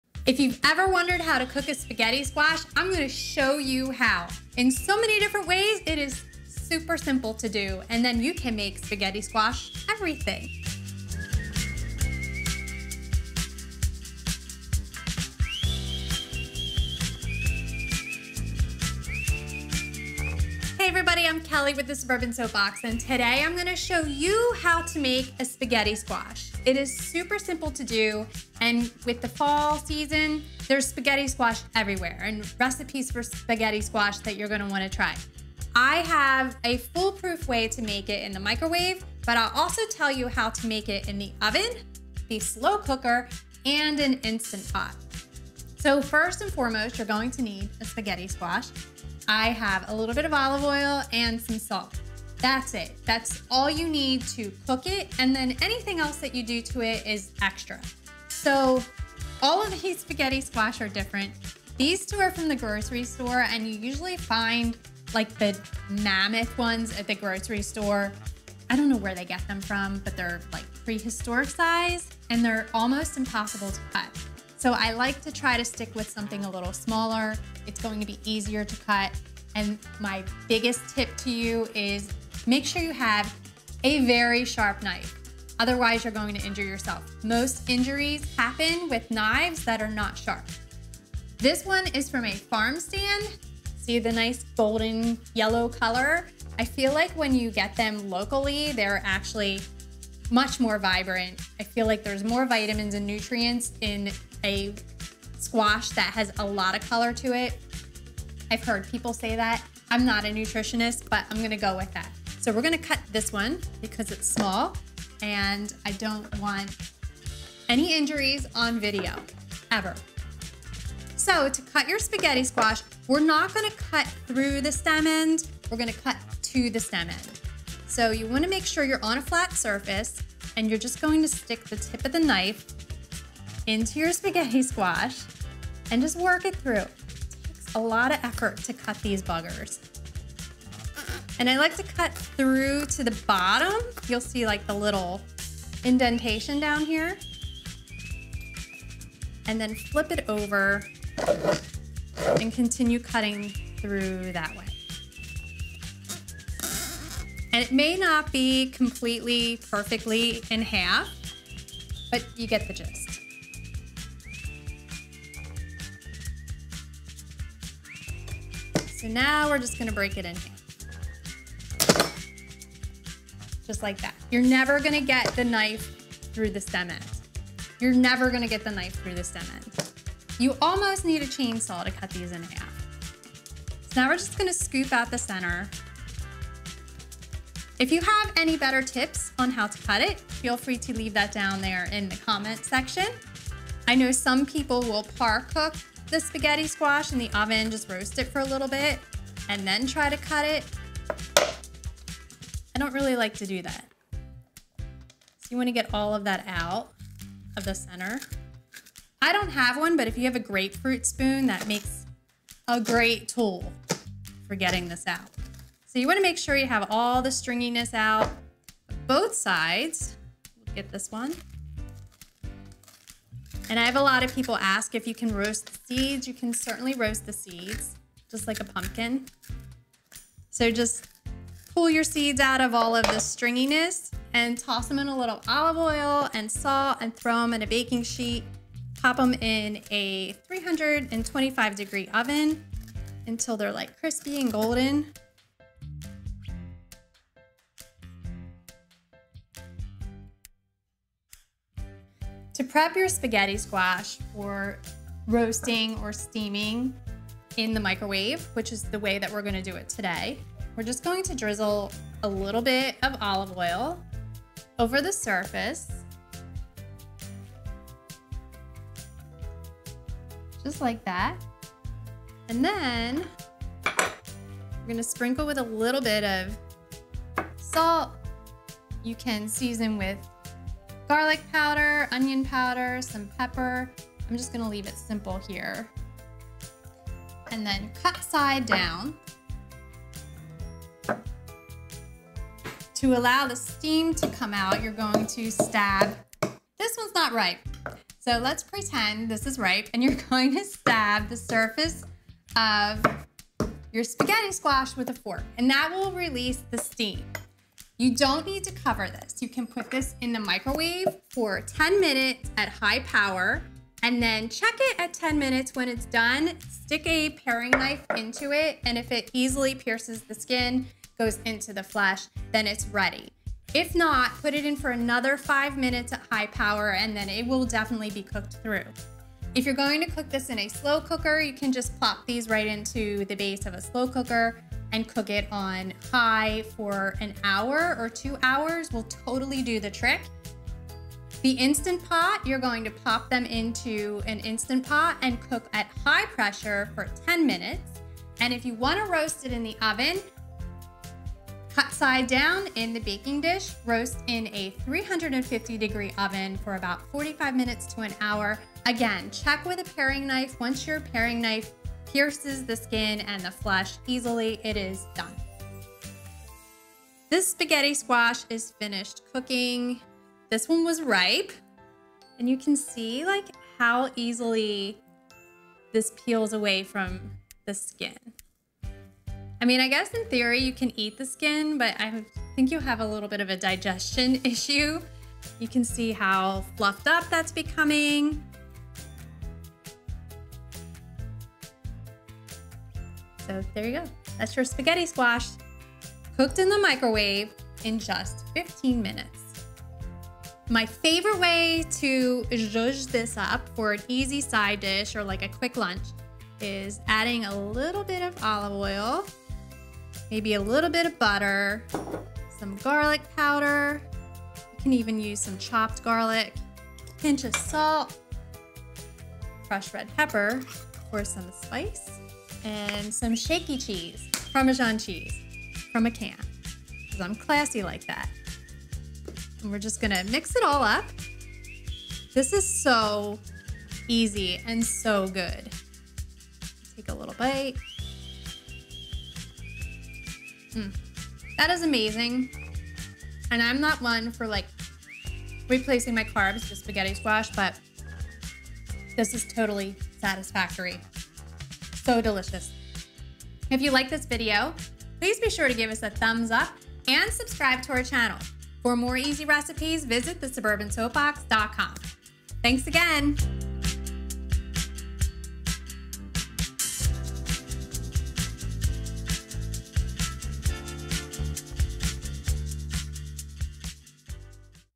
The EASIEST way to cook Spaghetti Squash in minutes!
So easy to make, Spaghetti Squash is healthy and filling and it's ready in just 10 minutes with the help of your microwave! This Spaghetti Squash recipe is as simple as it gets.
Ingredients
▢kosher salt and fresh ground pepper
Equipment
microwave
Instructions
Using a large, sharp knife, carefully cut the spaghetti squash in half lengthwise through the stem end.
Scoop out the seeds with a spoon and discard. (Or save the seeds and roast them.)
Drizzle the cut side of the squash with olive oil. Season with salt and pepper.
Place the squash, cut side down, in a baking dish. Using a fork, pierce several holes in the skin so steam can escape.
Cook the squash in the microwave on high for 10-15 minutes or until the flesh is fork tender.
Allow the squash to rest until cool enough to handle. Using a fork, shred the flesh of the squash to create long spaghetti strands.
Drizzle with olive oil and serve or use in your favorite pasta recipe.